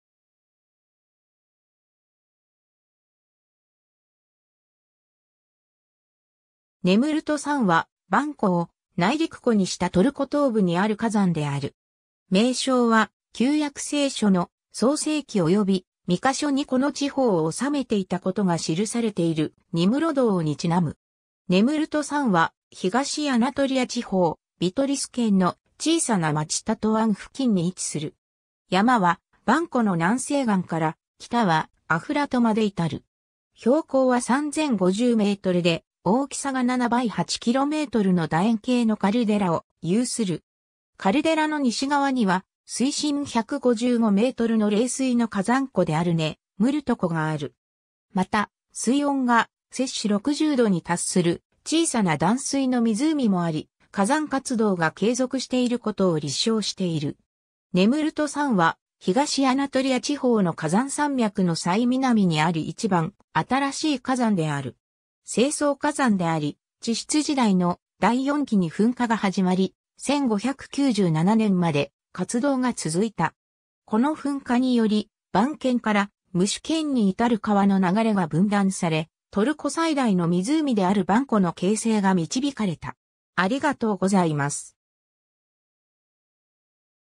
ネムルト山 (火山)
ネムルト山(トルコ語: Nemrut Dağı, アルメニア語: Սարակն Sarakn, "Mountain spring", クルド語: Çiyayê Nemrud)は、ヴァン湖を内陸湖にしたトルコ東部にある火山である。
名称は『旧約聖書』の「創世記」および「ミカ書」にこの地方を治めていたことが記されているニムロド王に因む。
 ネムルト山は東アナトリア地方ビトリス県の小さな町タトワン付近に位置する。
山はヴァン湖の南西岸から、北はアフラトまで至る。
 標高は3,050 m (10,007 ft)で、大きさが7 by 8 km (4 by 5 mi)の楕円形のカルデラを有する。
カルデラの西側には水深155 m (509 ft)の冷水の火山湖であるネムルト湖がある。
また、水温が60 °C (140 °F)に達する小さな暖水の湖もあり、火山活動が継続していることを立証している。
 ネムルト山は東アナトリア地方の火山山脈の最南にあり一番新しい火山である。
成層火山であり、地質時代の第四紀に噴火が始まり、1597年まで活動が続いた。
この噴火により、ヴァン県からムシュ県に至る川の流れが分断され、トルコ最大の湖であるヴァン湖の形成が導かれた。